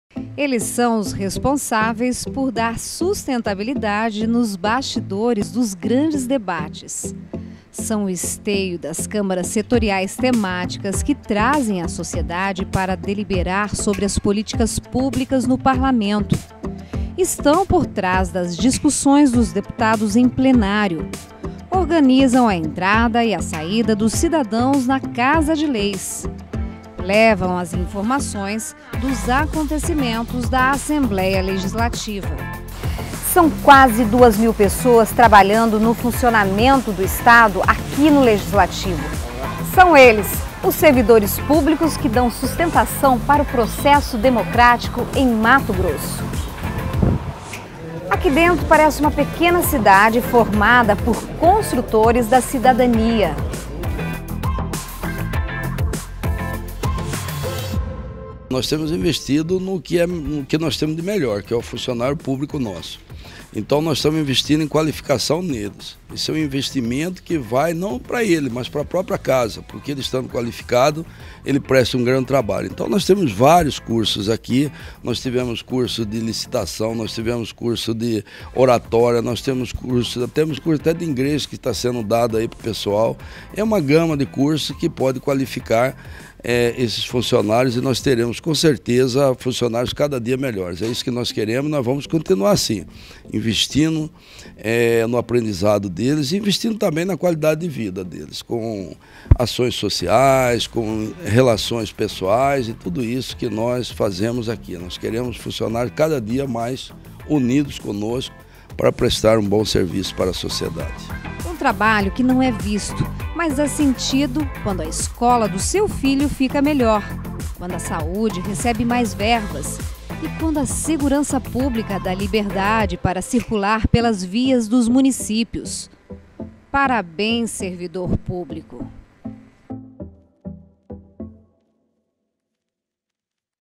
Servidores públicos contribuem para a democracia
O dia 28 de outubro, é dia do servidor público. A data foi constituída no governo de Getúlio Vargas, ainda em 1937. O objetivo é homenagear esses profissionais que trabalham em prol do bem comum em todas as esferas e poderes, inclusive aqui no parlamento estadual.